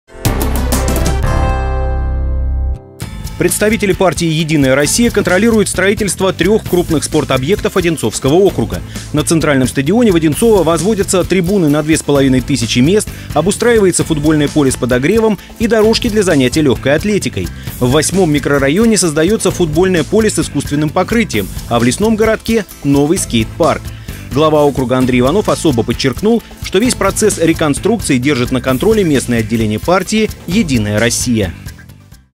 Единороссы контролируют стройки
Представители партии «Единая Россия» контролируют строительство 3-х крупных спортобъектов Одинцовского округа. На центральном стадионе в Одинцово возводятся трибуны на 2500 мест, обустраивается футбольное поле с подогревом и дорожки для занятия лёгкой атлетикой.